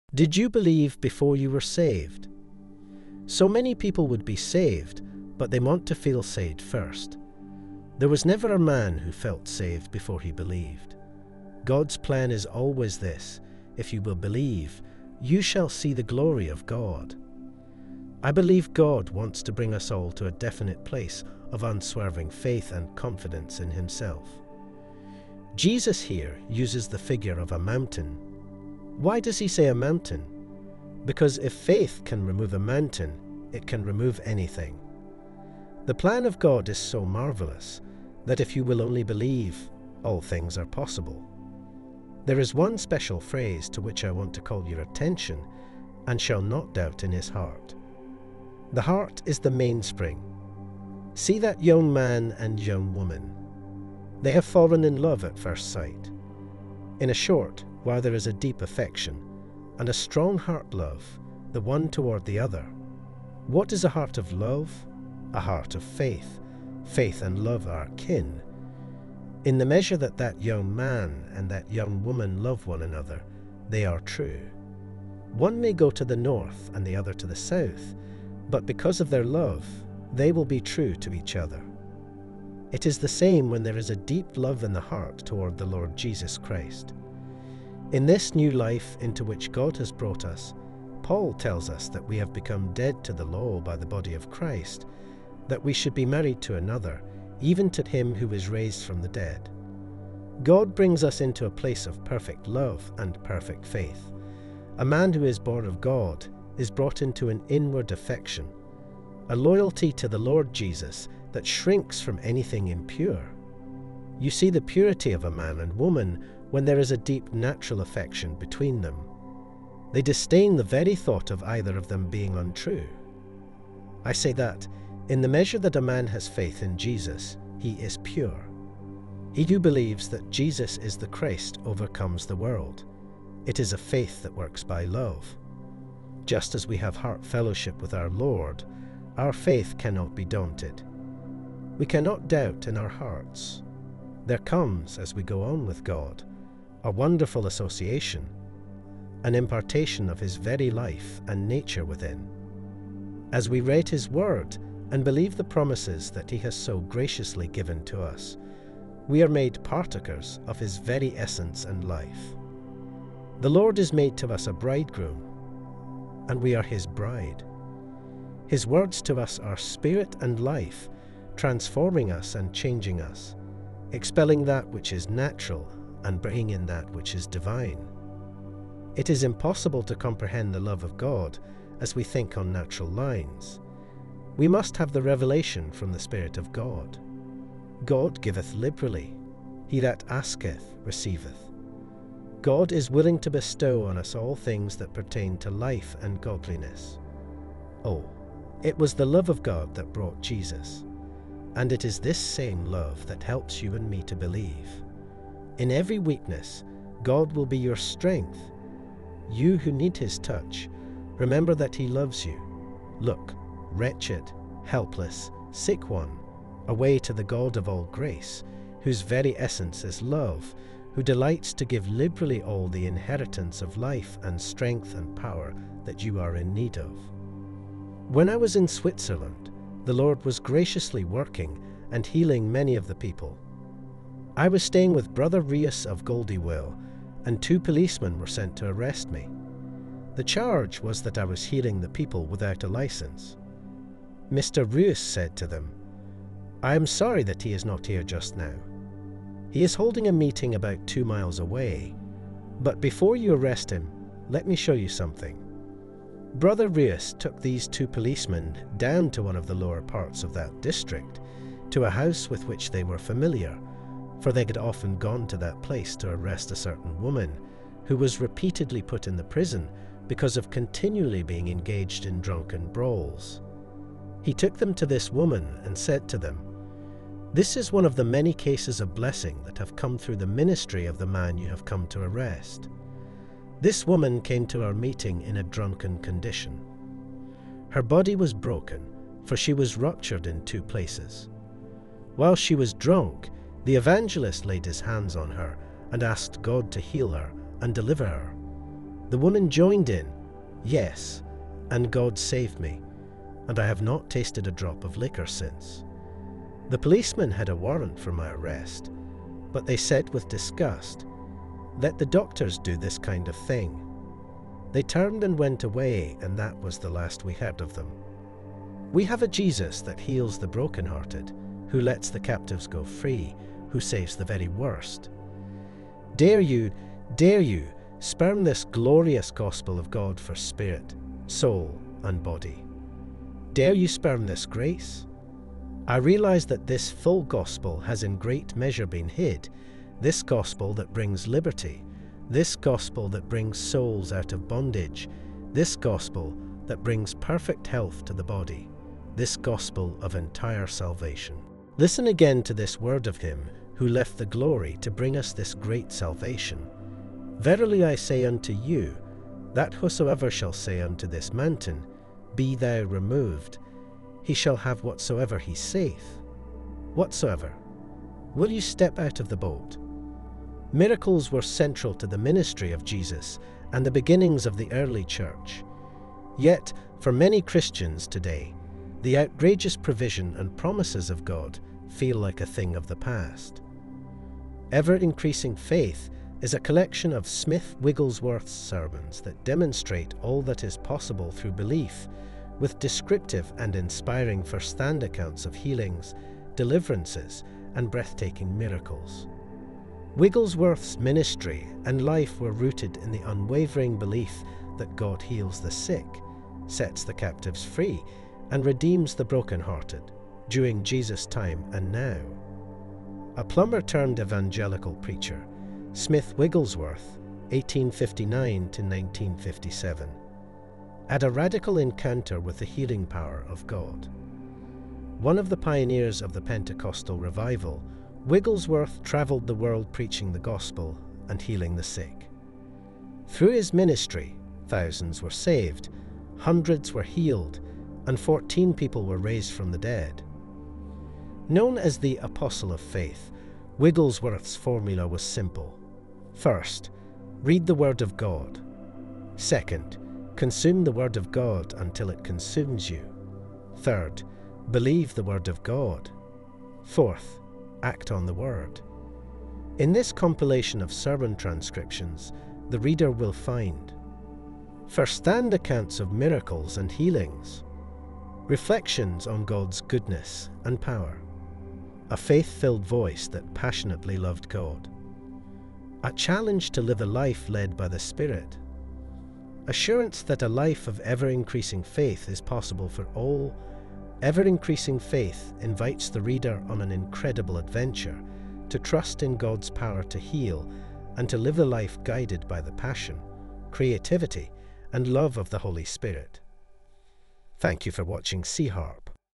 How to Move Mountains with Your Heart | Smith Wigglesworth
"Ever Increasing Faith" is a collection of sermons by Smith Wigglesworth, a plumber-turned-evangelical-preacher known as the "Apostle of Faith." In these sermons, Wigglesworth shares inspiring firsthand accounts of miracles, healings, and breathtaking acts of God's power. He believed in the unwavering ability of God to heal the sick, set captives free, and redeem the brokenhearted, both in the time of Jesus and in the present. Wigglesworth's formula for a life of ever increasing faith is simple: read, consume, believe, and act on the Word of God. This compilation offers reflections on God's goodness and power, a faith-filled voice that passionately loved God, and a challenge to live a life led by the Holy Spirit. It invites readers on an incredible adventure to trust in God's healing power and live a life guided by the Spirit's passion, creativity, and love.